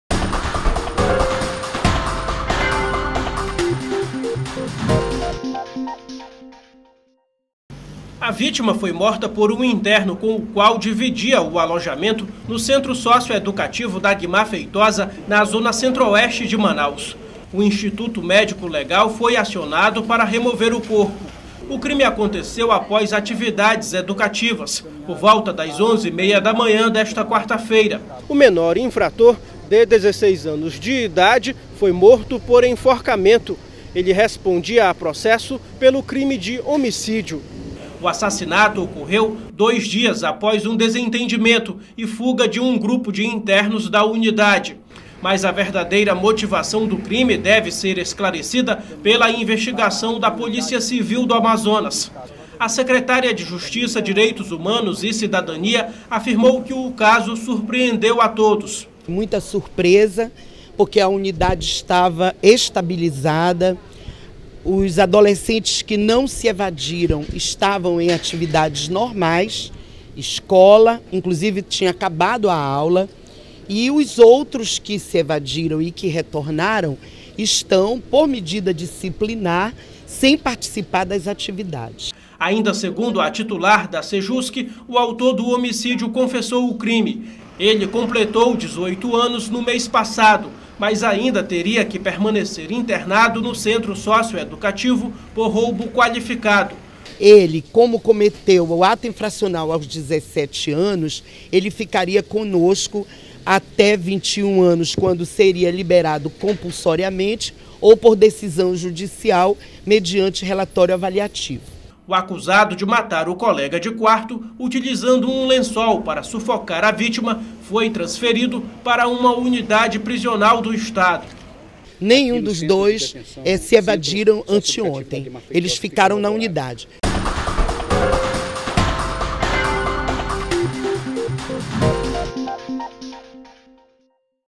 Adolescente de 16 anos é morto dentro do centro Dagmar Feitosa
Um adolescente de 16 anos foi assassinado dentro do centro socioeducativo Dagmar Feitosa, na zona centro-oeste de Manaus, onde cumpria internação por ter cometido um homicídio. O suspeito de cometer o crime contra o adolescente já possui 18 anos e foi transferido para uma unidade prisional.